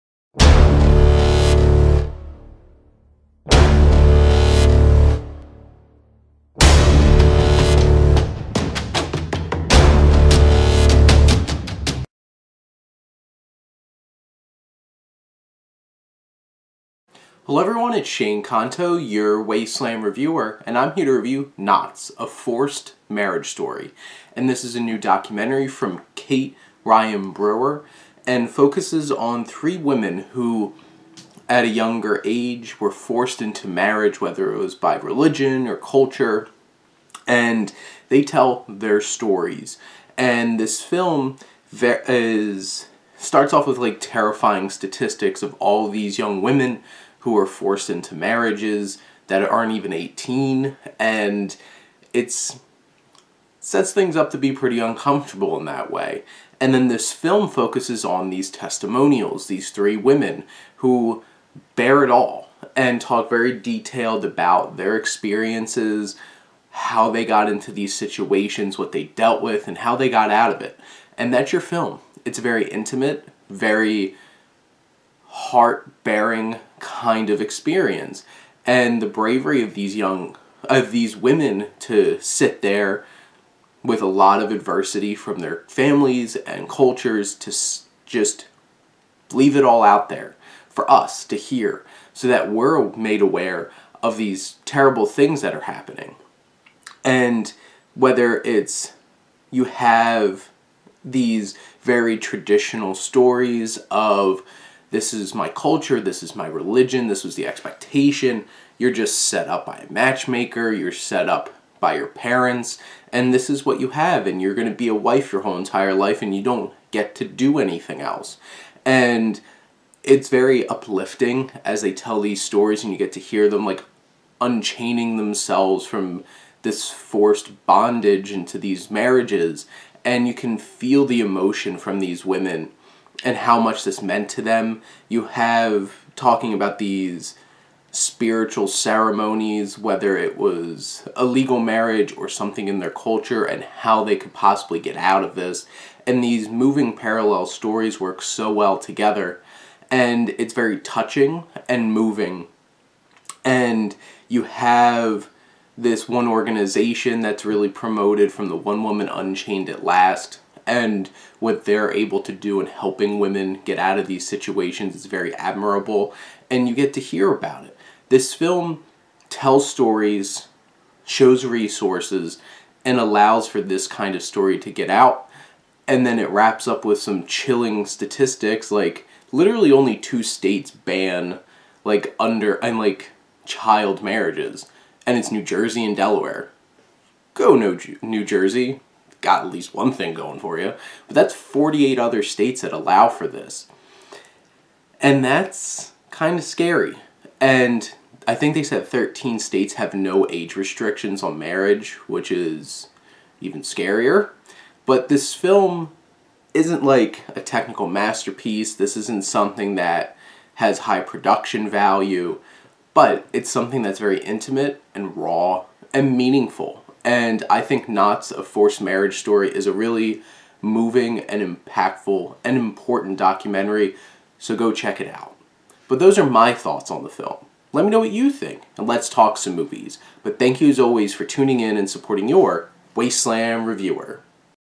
Knots: A Forced Marriage Story Wasteland Review
My Wasteland Review of this new documentary about three women's stories about being forced into marriage at a young age, Knots: A Forced Marriage Story.